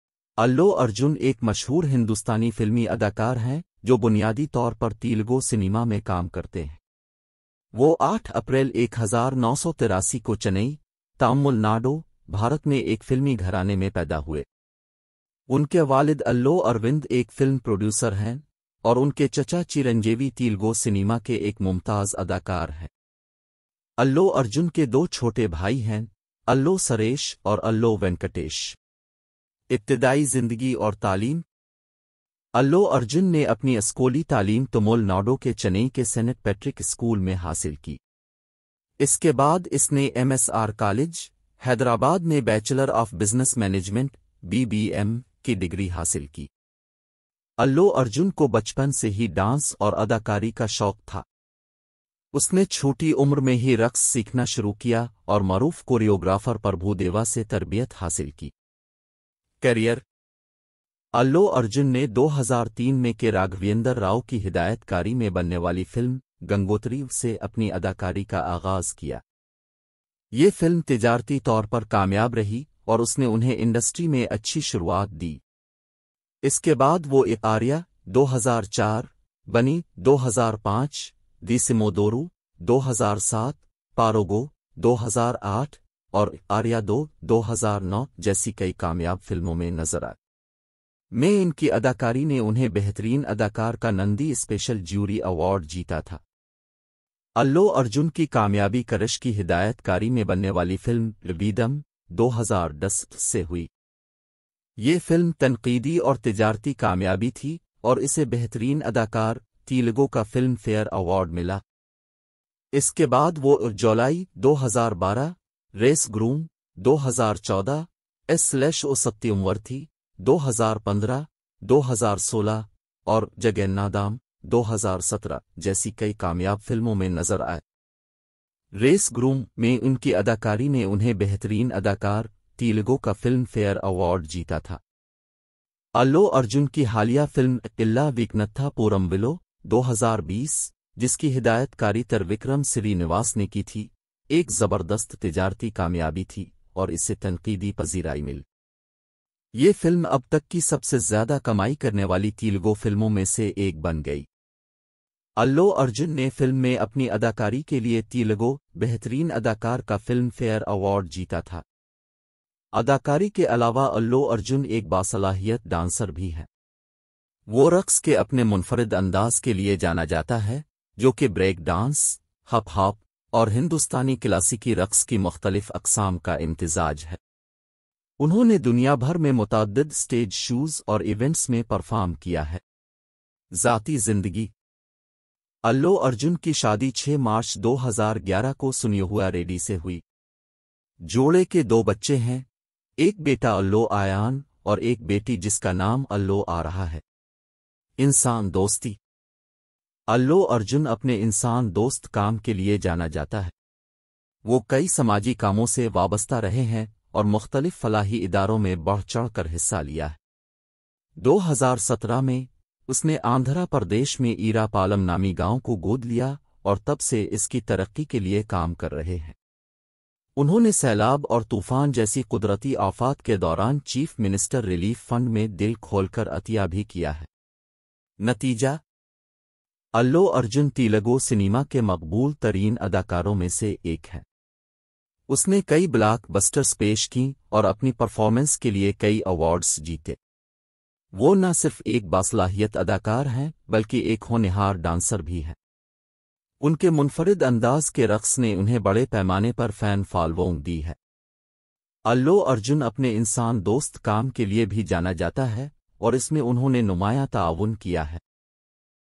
Allu Arjun Incredible Journey of Superstar Biography of Allu Arjun In Urdu
Hello, Everyone!
Welcome to the world of Some Little Joy. This video is about to Happy videos...

Allu Arjun Incredible Journey to Superstar Biography of Allu Arjun In Urdu
Allu Arjun Incredible Journey to Superstar biography of alluarjun In urdu